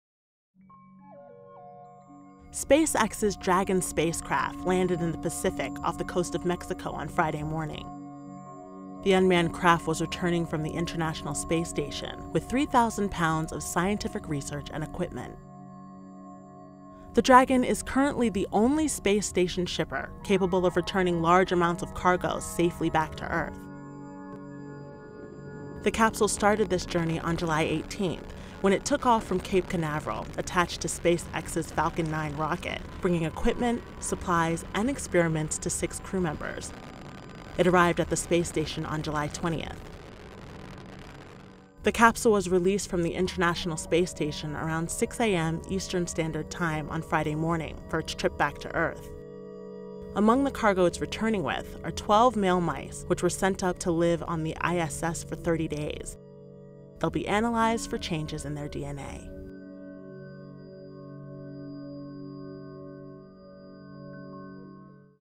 SpaceX's Dragon Capsule Returns to Earth
SpaceX's Dragon spacecraft, which landed in the Pacific off the coast of Mexico on Friday morning, is the only vessel currently capable of returning large amounts of cargo safely back to Earth from the International Space Station. Photo: NASA